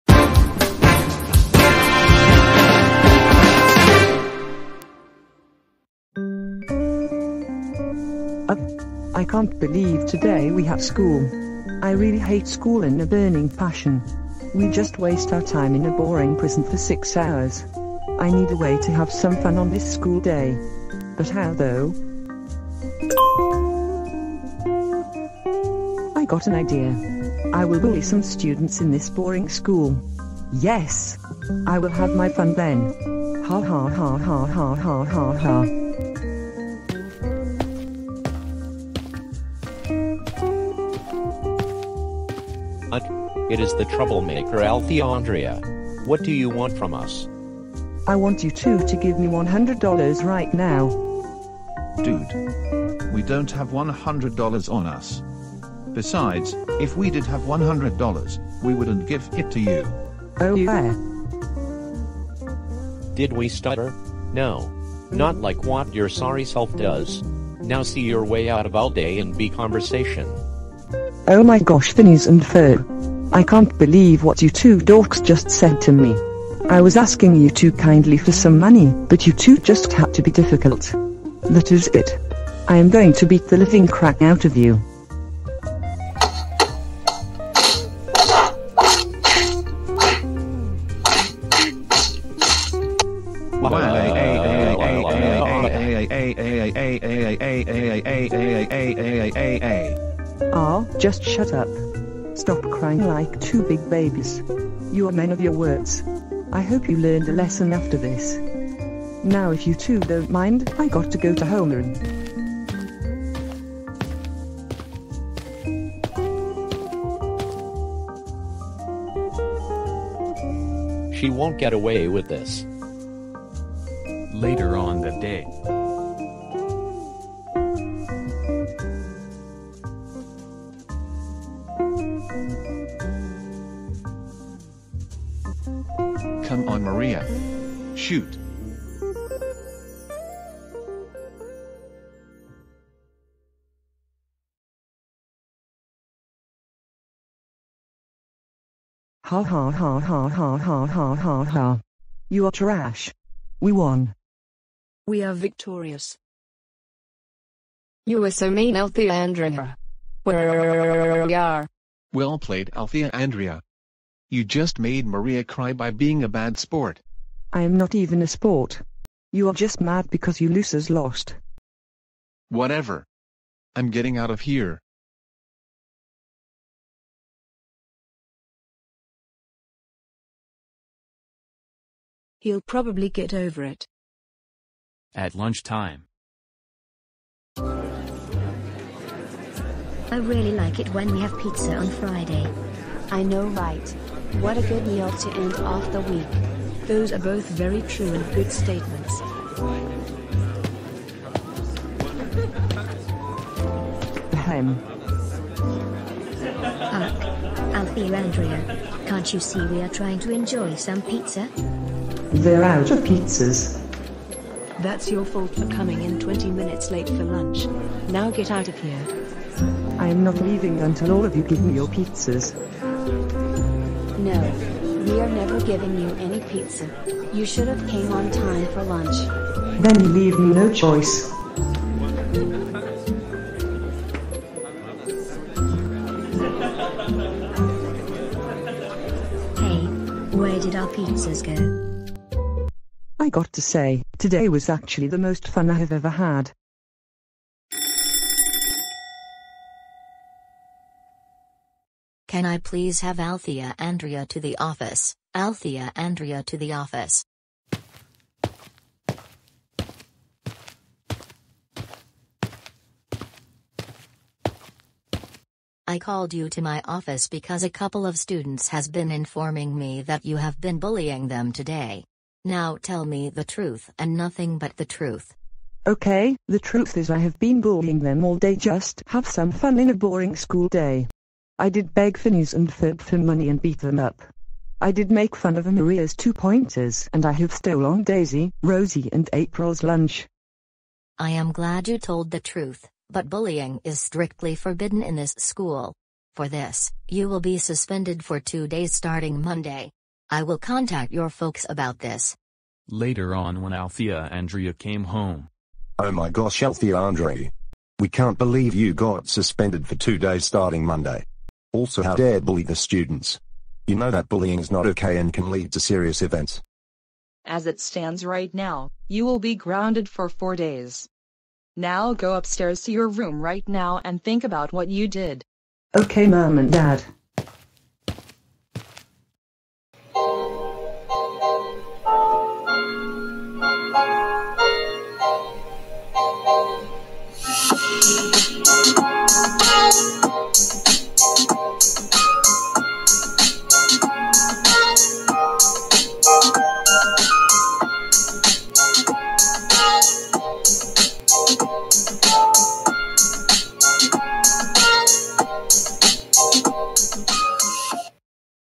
Althea Andrea bullies Students and Gets Grounded
This is my first Grounded video out of Althea Andrea.
Looks like Althea Andrea wants a little fun.